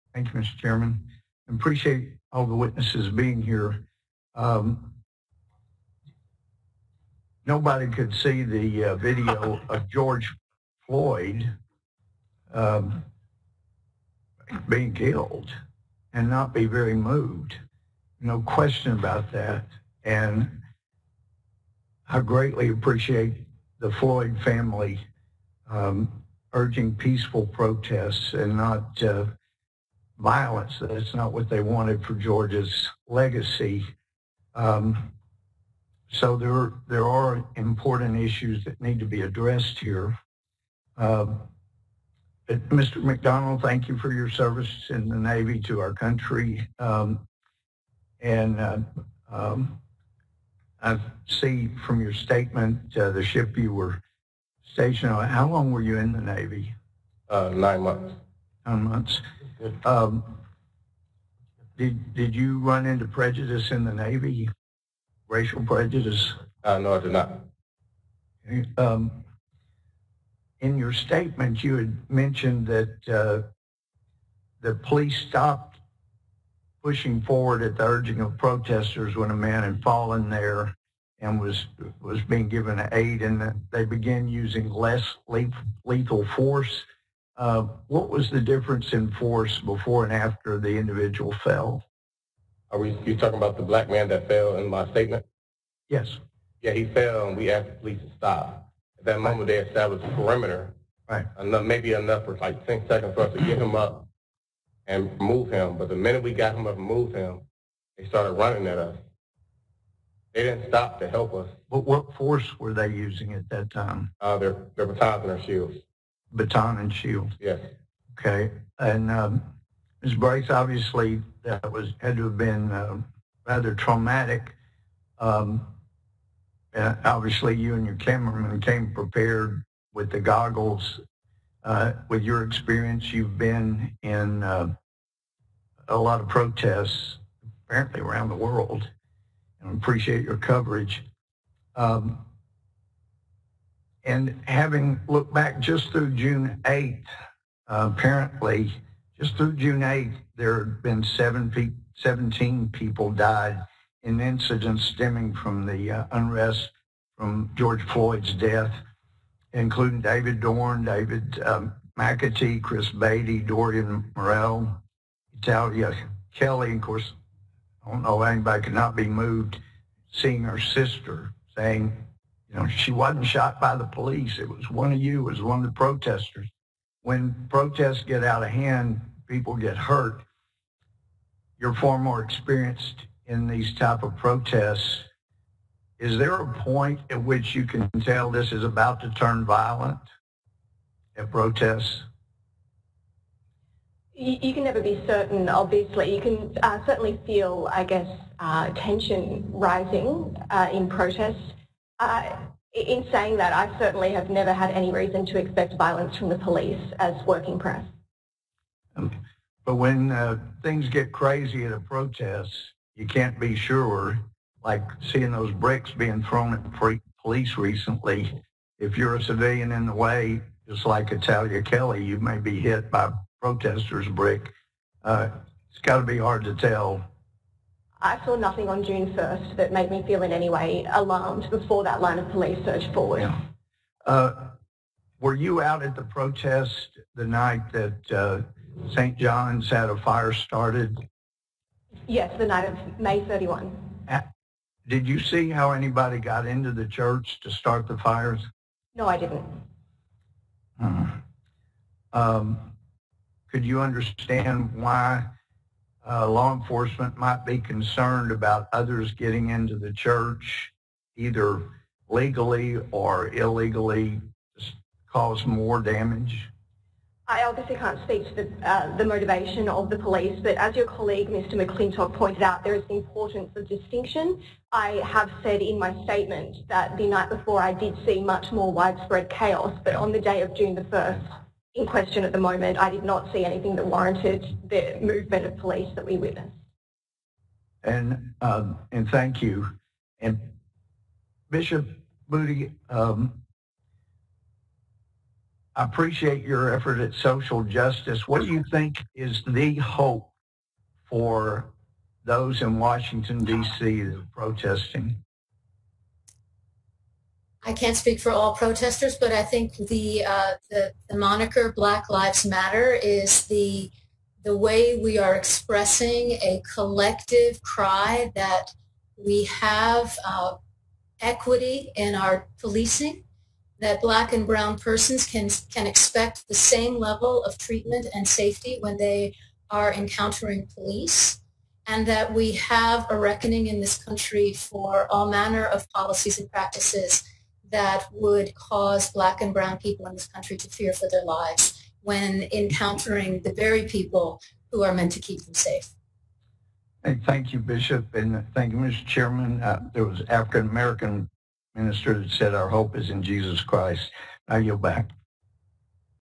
Gohmert Questions Witnesses on Recent Protests in Lafayette Square
Congressman Louie Gohmert (TX01) spoke in a House Natural Resources Committee oversight hearing titled, "The U.S. Park Police Attack on Peaceful Protesters at Lafayette Square." He questioned witnesses on the nature of the protests that have stemmed from the unrest following George Floyd's death. He noted in the hearing, "When protests get out of hand, people get hurt."